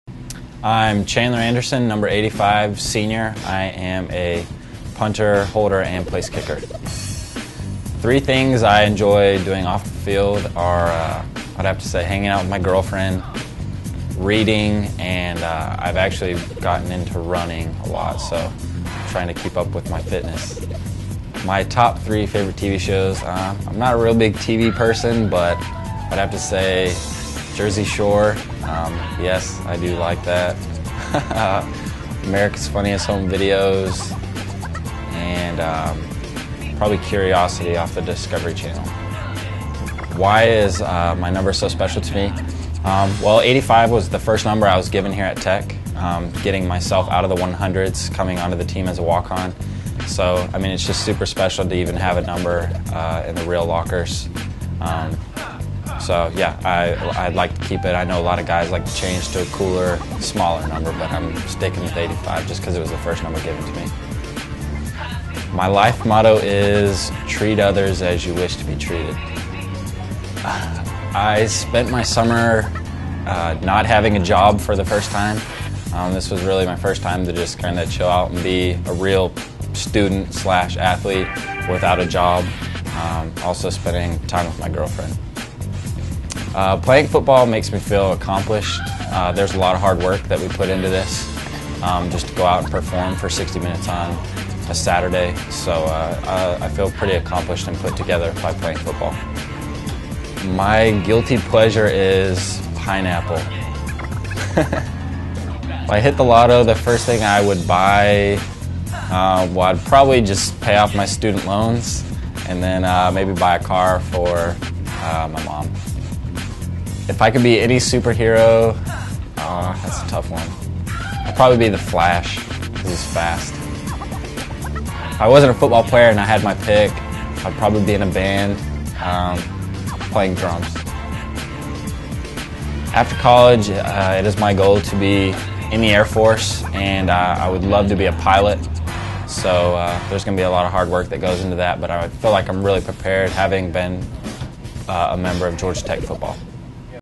Chandler Anderson 10 Questions
Georgia Tech Kicker/Punter Chandler Anderson answers 10 questions for Yellow Jacket fans